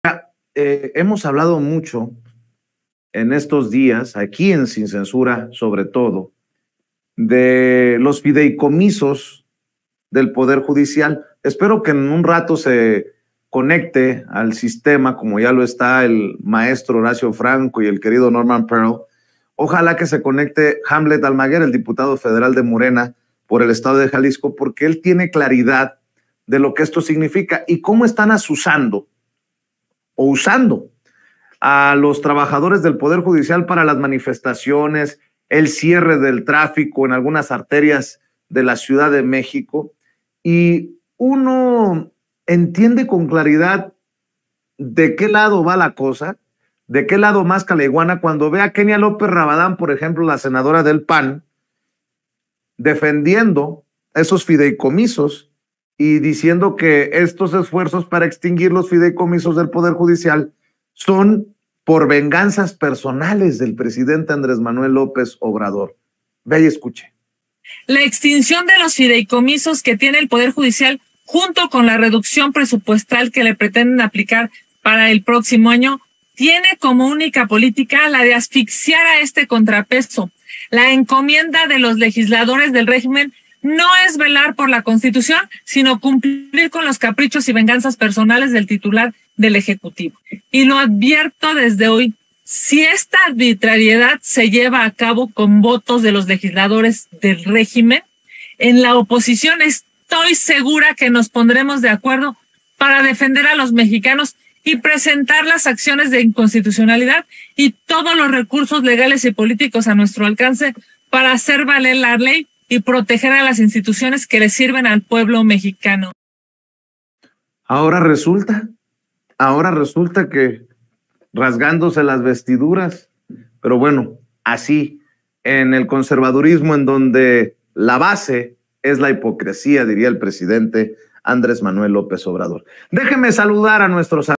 OPOS1C1ON “LL0RA”: BUSC4N FREN4R EXTINCIÓN DE FIDEICOMISOS
Kenia López Rabadán, vicecoordinadora del PAN, adelantó que, de concretarse la extinción de fideicomisos y el recorte presupuestal al Poder Judicial de la Federación, la “oposición se unirá” para presentar acciones de inconstitucionalidad